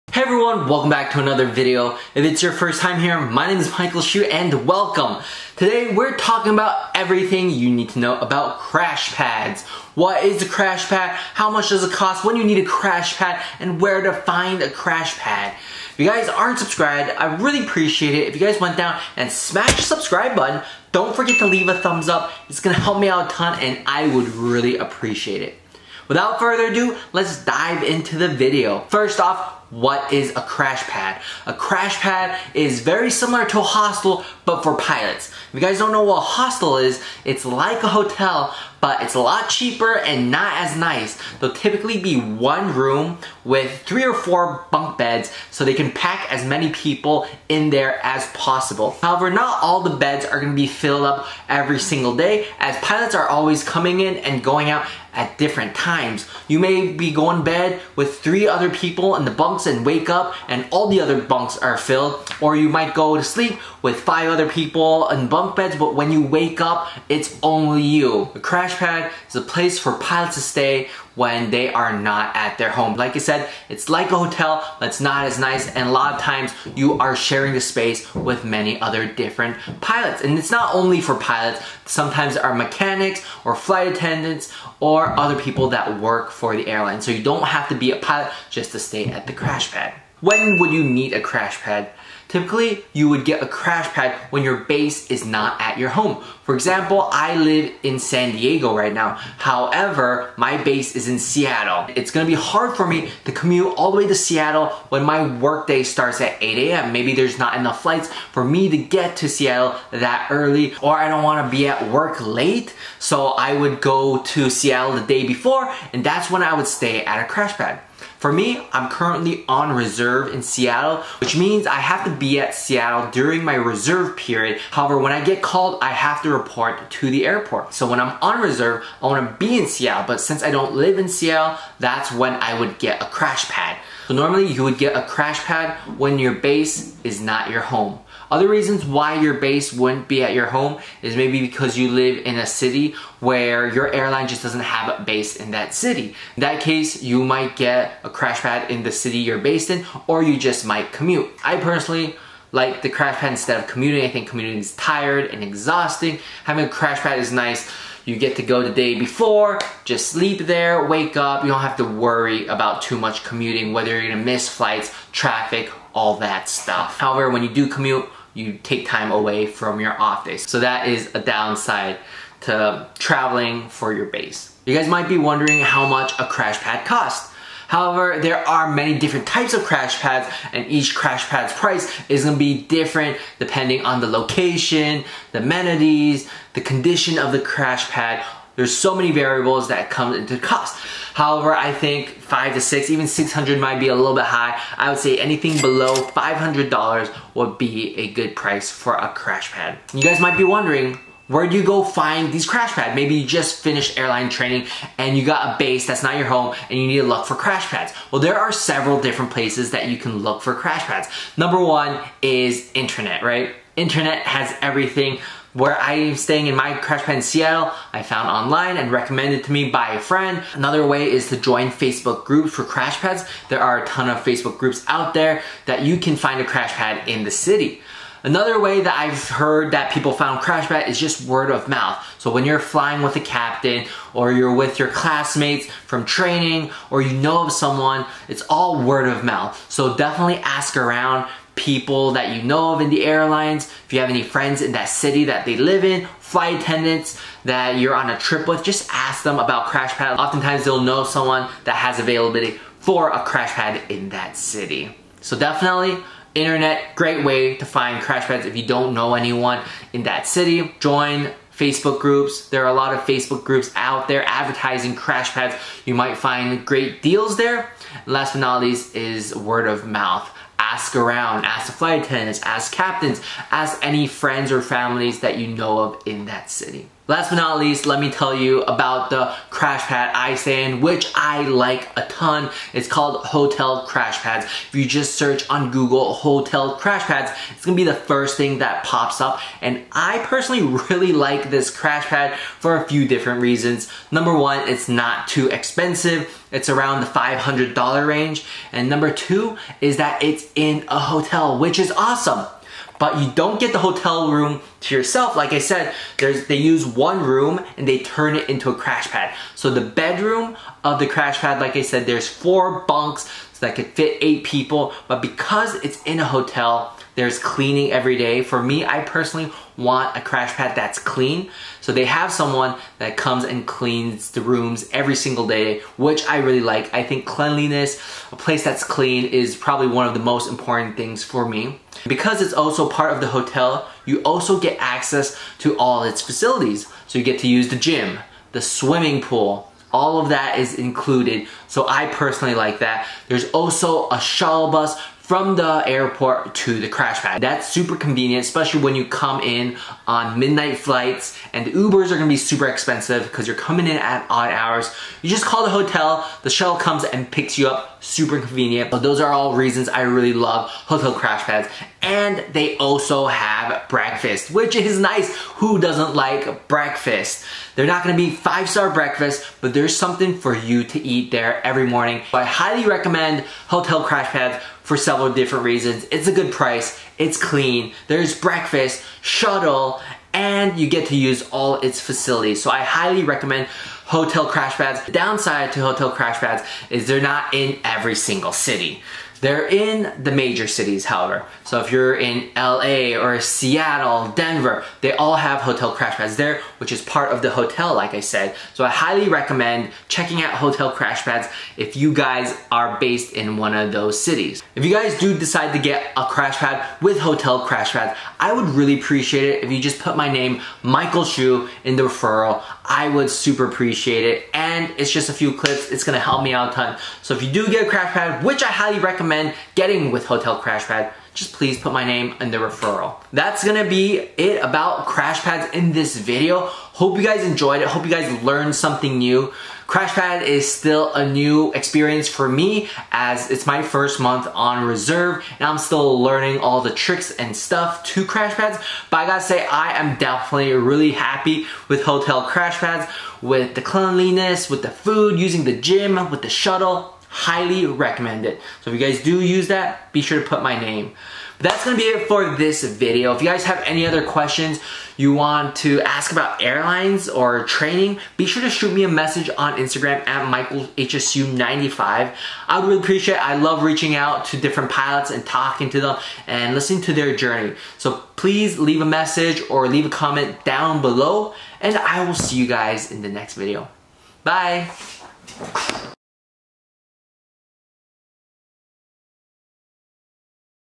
Everything You Need To Know About Crash pads
In this video we talk about everything you need to know about crashpads. Where to find one, how much they cost and what to expect. Hope this video helps someone. and be sure to put my name down in the referral if you do use hotel crashpad